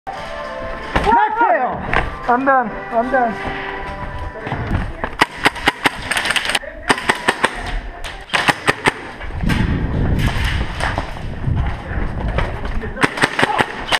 Random Airsoft Clips #1
Just looking through videos and clipped these together.